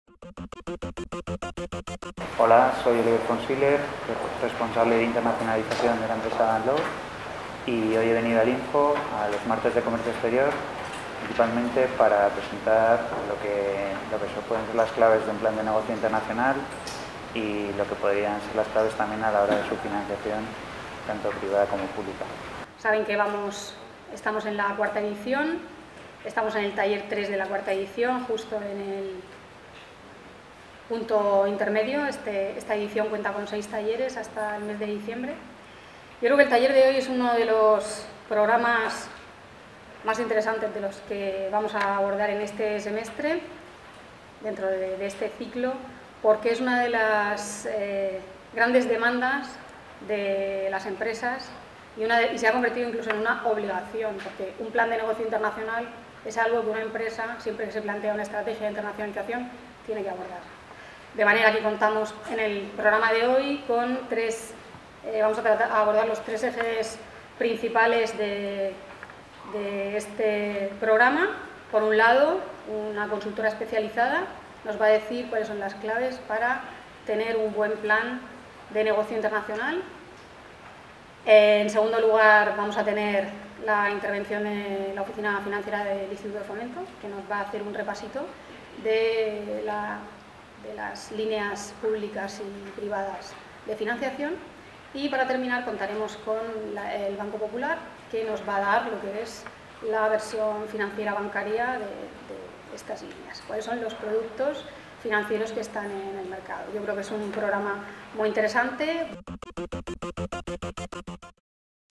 Cómo preparar un buen Plan de Negocios Internacional y su financiación (Versión INFO, 4 nov 2014)
Tercer taller de la 4ª edición de "Los Martes del Comercio Exterior del INFO". La jornada trata sobre "Cómo preparar un buen Plan de Negocios Internacional y conseguir financiación", participaran como ponentes: AD & LAW consultores,  Entidades bancarias
- Mª José Bernal Oficina Financiera INFO. Más información